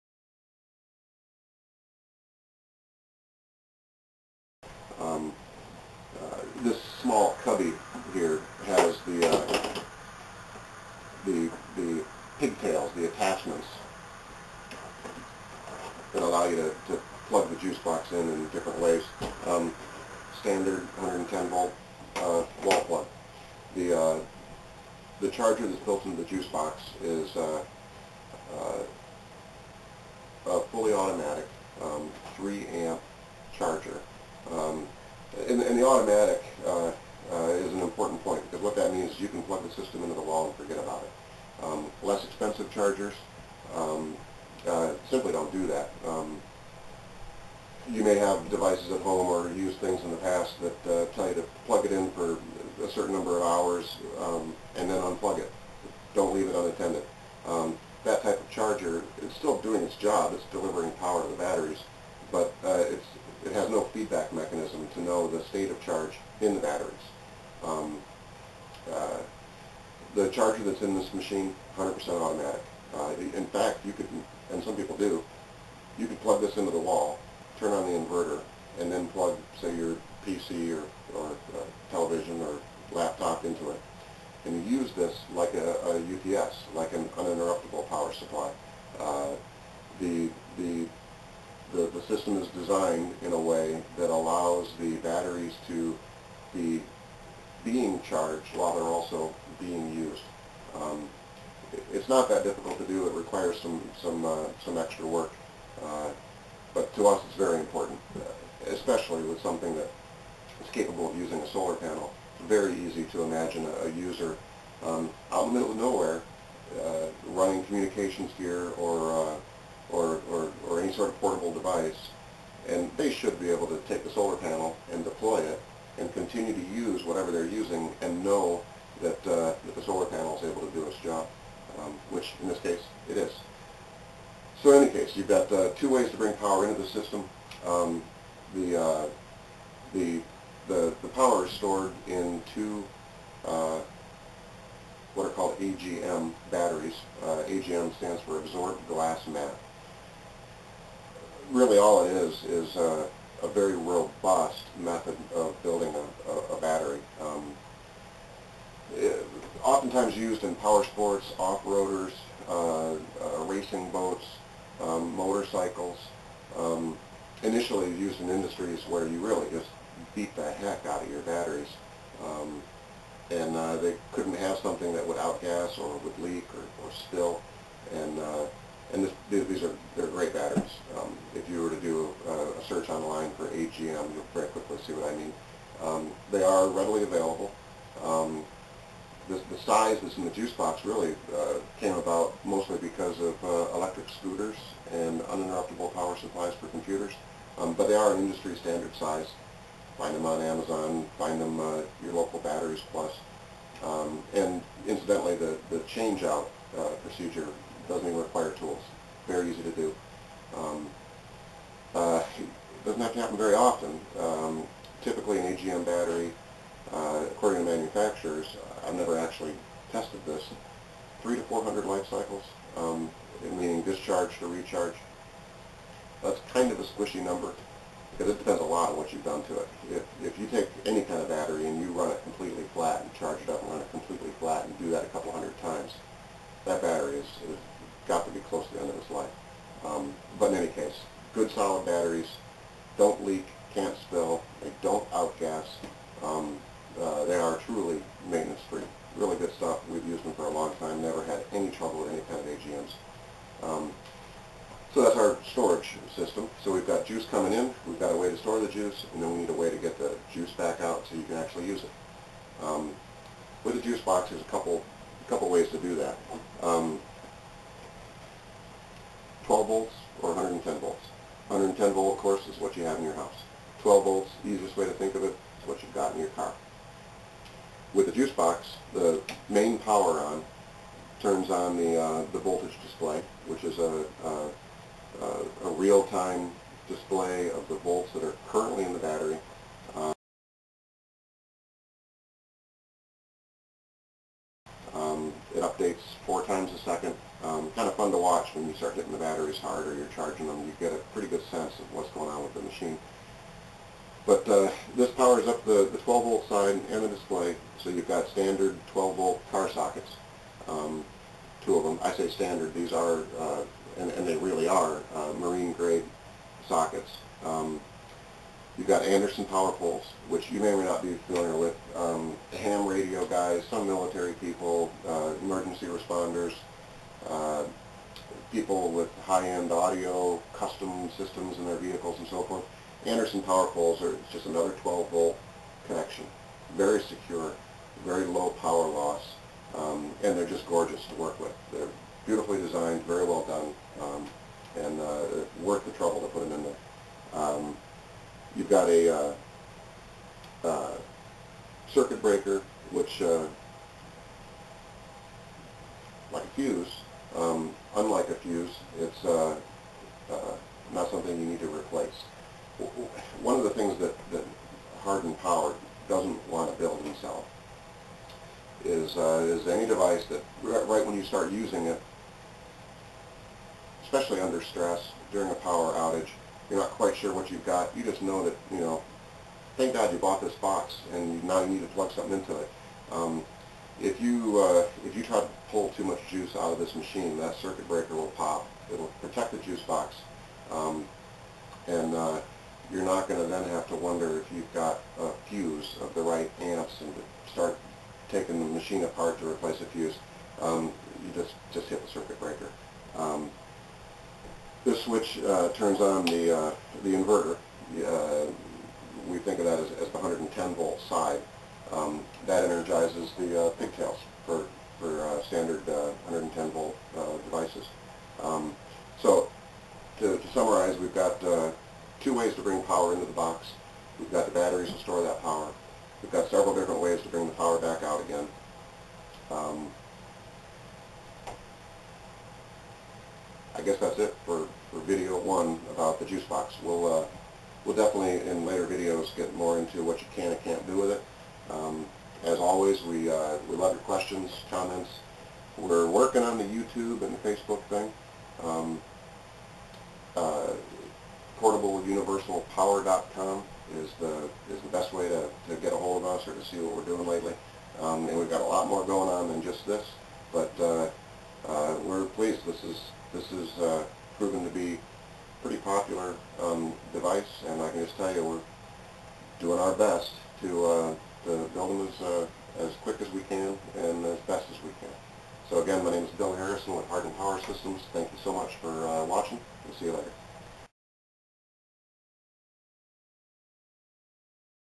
2 JuiceBox- briefcase sized waterproof, uninterrupted power, rechargeable, self-sustaining
An introduction of the JuiceBox.  A complete Alt-Energy system in a can!  Hardened Power Systems is the designer, manufacturer, and wholesaler of this device.  We also design and build custom solutions to unusual energy problems...Imagine a briefcase sized waterproof box that can silently power your PC, laptop, radio, fans, small television, and more. it will easily charge your phone, Ipad, tablet... any common portable device. When it needs recharging, simply plug it into the wall or use the included solar panel. No gas, no fumes, no noise.

This unit is very popular with campers, hunters, boaters, radio operators and preppers.

Of course, anyone who has learned the painful lessons of natural disasters or long periods of power loss appreciates how valuable self-sustaining power can be...

If you have medical devices that need uninterrupted power this may be a solution, especially if combined with a JuiceBrick. If you have medical needs, please call us or email so we can discuss the specific power requirements of your equipment and how you want to use it.
On a single charge, the JuiceBox can:
Charge a typical laptop six times
Charge a Smart Phone 50 times
Charge a Cell Phone 80 times
Run an I-Pad® for nearly 100 hours
Run a portable radio for weeks
Run a GPS for 140 hours
Run a desktop fan for 20 hours
Light up a campsite for several days